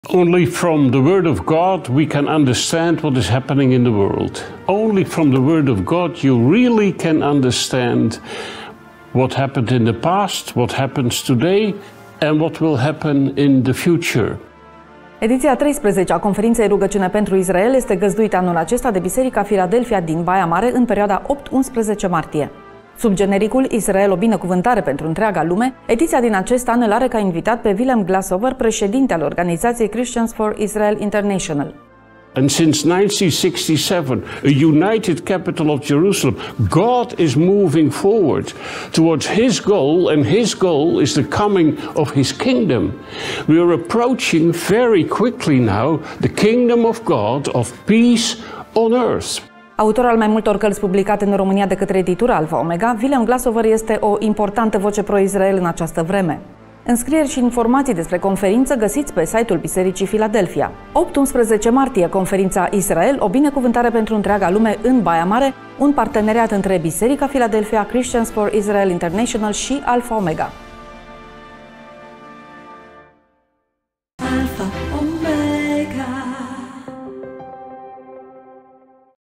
Conferința „Israel, o binecuvântare pentru întreaga lume”
Faci parte dintr-o generație specială! 👐
Știm că nu mai e mult și Dumnezeu va duce planul Său de răscumpărare la desăvârșire.

🕑 Acum e momentul să cunoști mai multe despre
• Întoarcerea evreilor în Israel
• Treziri spirituale în țările musulmane
• Situația actuală din Orientul Mijlociu

Vino la conferința: „Israel, o binecuvântare pentru întreaga lume”.
🗓️ 8-11 Martie
📍 Biserica Filadelfia, Baia Mare

Invitat: 🗣️ Rev. Willem J.J. Glashouwer

Eveniment organizat de Alfa Omega TV în parteneriat cu Biserica Filadelfia Baia Mare și Christians for Israel International.
O parte dintre cărțile scrise de Willem J.J. Glashouwer sunt disponibile în Librăria Alfa Omega.